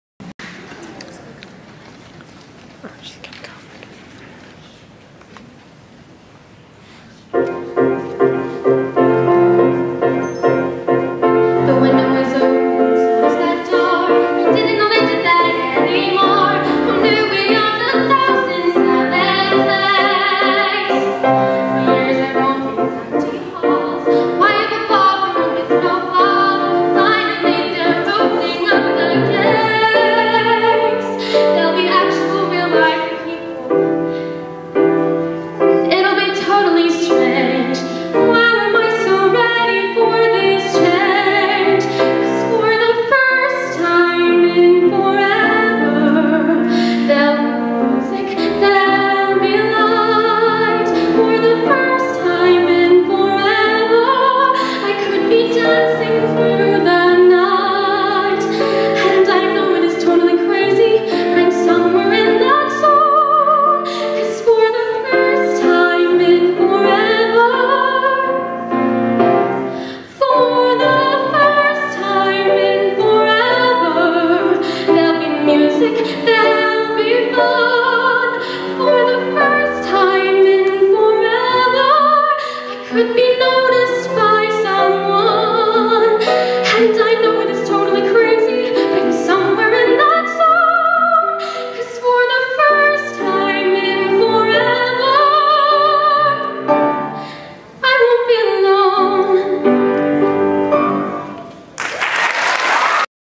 Dakota Finley Sings!
My daughter Dakota singing a solo at her final Spring Choral Concert